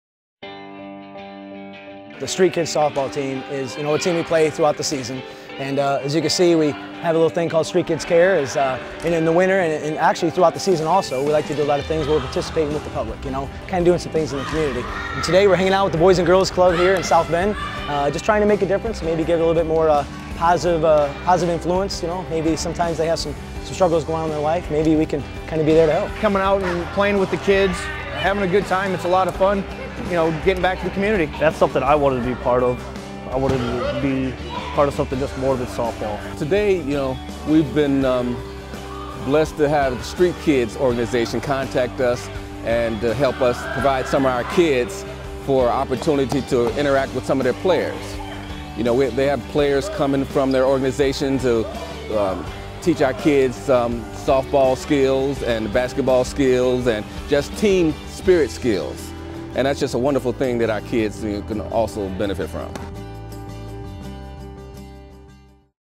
skcareboysgirlsclub
The Street Kids Softball team spent time with the Boys and Girls Club of South Bend on Saturday, December 20th, 2008.